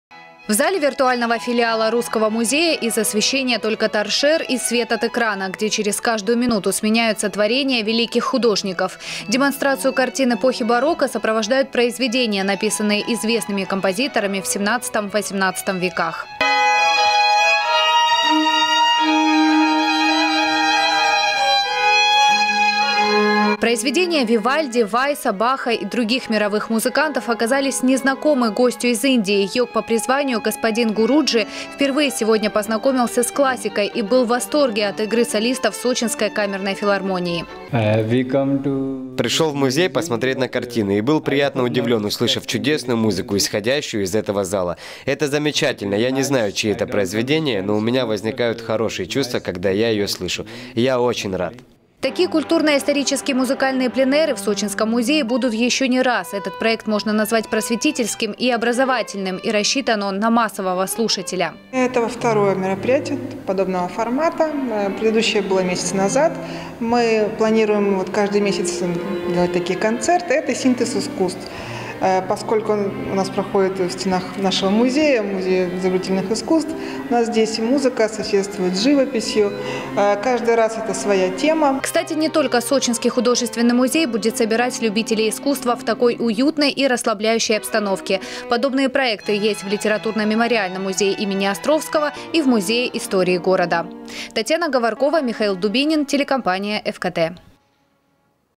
В Сочинском художественном музее сегодня совместили два вида искусства. Новости Сочи Эфкате
Музыка и живопись слились в музыкальном пленэре. Цикл таких концертов стартовал на курорте месяц назад. Тогда сочинцы и гости курорта могли познакомиться с искусством эпохи ренессанса и русской музыкой, а сегодня посетители окунулись в атмосферу «барокко».
Сочи. Новости.
Телеканал Эфкате.